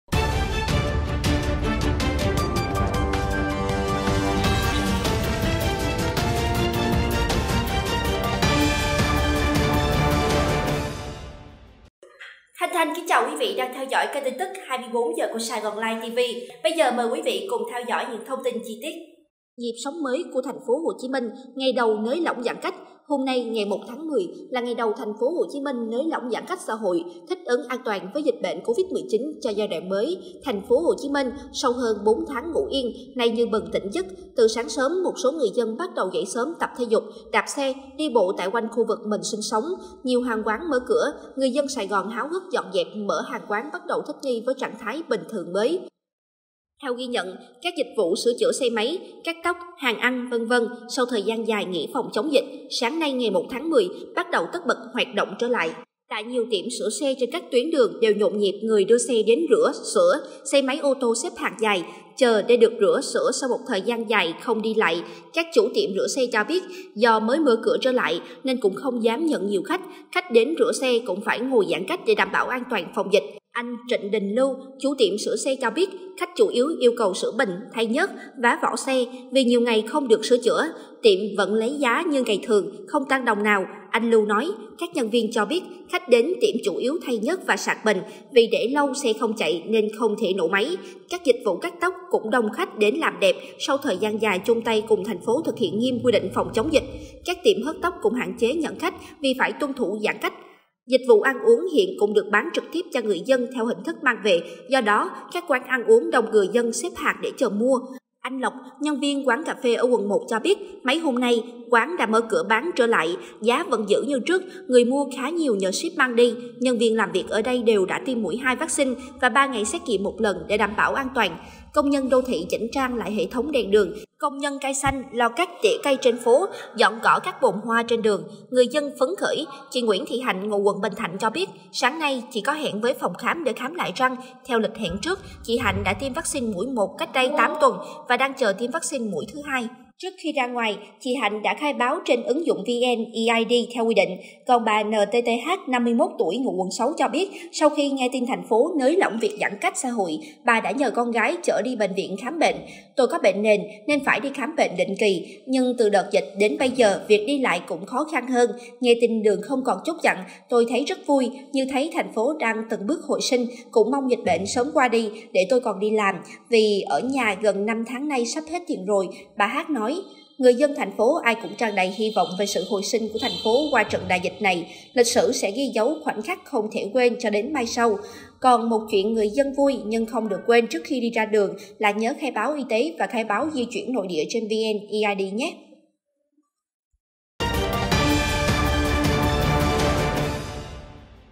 NHỊP SỐNG MỚI CỦA TPHCM NGÀY ĐẦU NỚI LỎNG GIÃN CÁCH | TIN TỨC 24H | SAIGON LIFE TV
Tp . HCM sau hơn 4 tháng ngủ yên nay như bừng tỉnh giấc.
Từ sáng sớm, một số người dân bắt đầu dậy sớm tập thể dục, đạp xe, đi bộ tại quanh khu vực mình sinh sống.
Nhiều hàng quán mở cửa, người dân Sài Gòn háo hức dọn dẹp, mở hàng quán bắt đầu thích nghi với trạng thái 'bình thường mới'.
Theo ghi nhận, các dịch vụ sửa chữa xe máy, cắt tóc, hàng ăn,... sau thời gian dài nghỉ phòng, chống dịch, sáng nay (1/10) bắt đầu tất bật hoạt động trở lại.